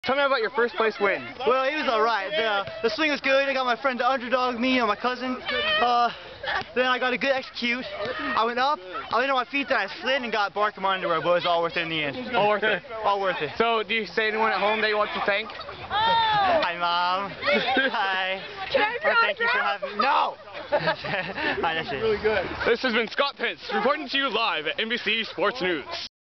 At Your House - After-Swing Interview
Scott interviews Dustin directly after jumping off the swing. EVERYTHING HE SAYS IS THE TRUTH! BELIEVE IT!